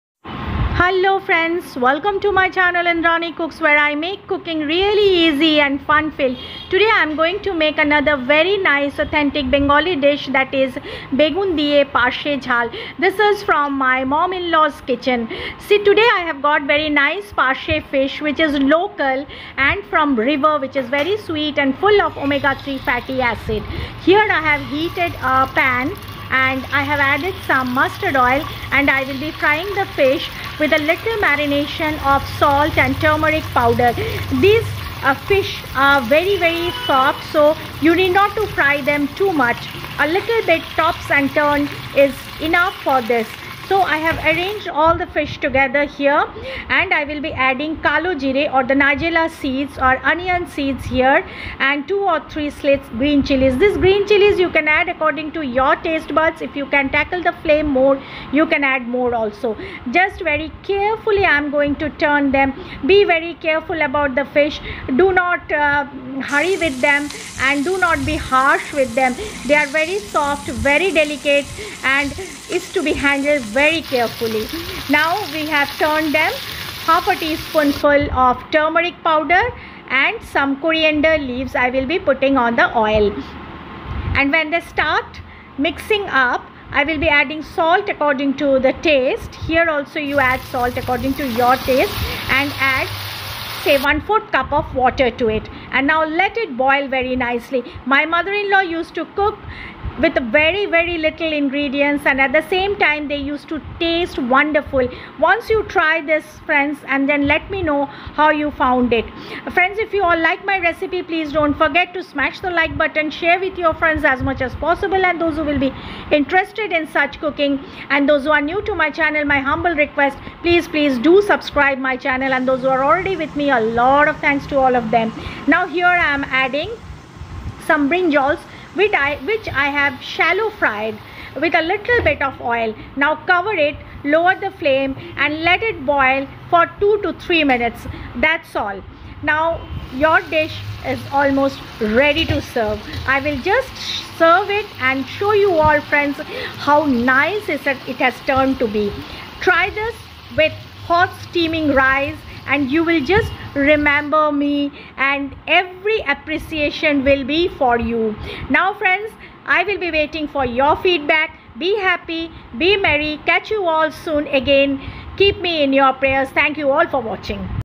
Parshe machher begun jhal || পারশে মাছের বেগুন ঝাল
Kasundi link

you can pick from upur local market also . it's easily available.

Ingredients

Parse fish
musturd oil
kalo jire (Nigella seeds)
Green chillies
Turmeric powder
Salt to taste
Coriander leaves
Shallow fried brinjal 🍆

Parshe is slightly sweet, freshwater fish, having very oily skin and a great flavour. It contains a lot of nutrients. It is good for your heart and brain.

Parshe Fish is loaded with important nutrients, such as protein and vitamin D. It is also a good source of omega-3 fatty acids, which are very important for the functioning of your body and brain.